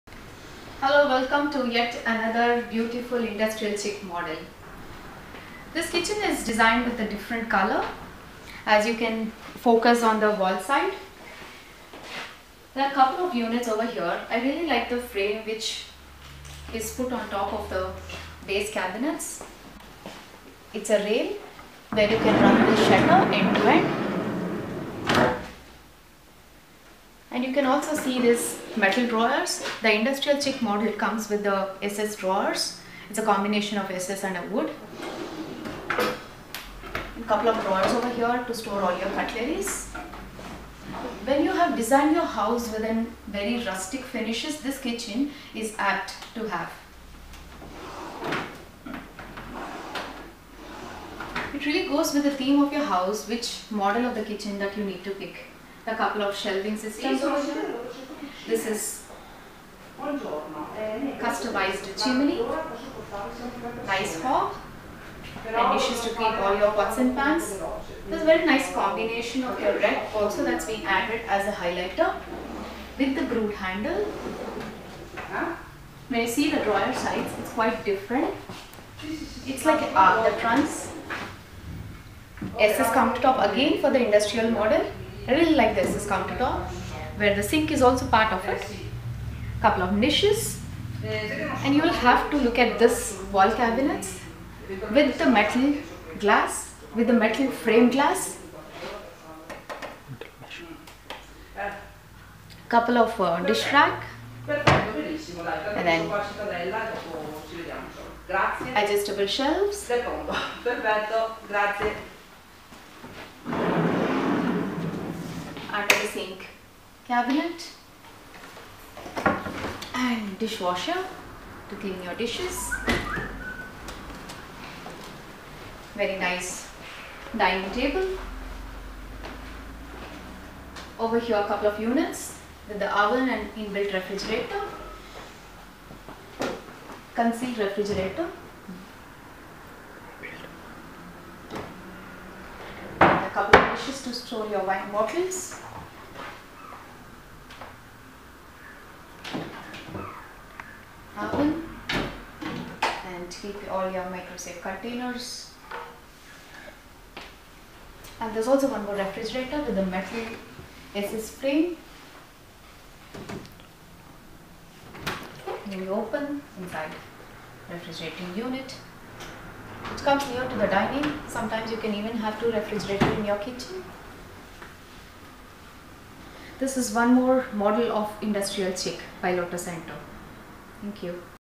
Lottocento Industrial Kitchen Style 2 - Solana Design Studio
Its great time to shift over to an industrial-style kitchen...Cutting across generations, it is a trend that never seems to die down!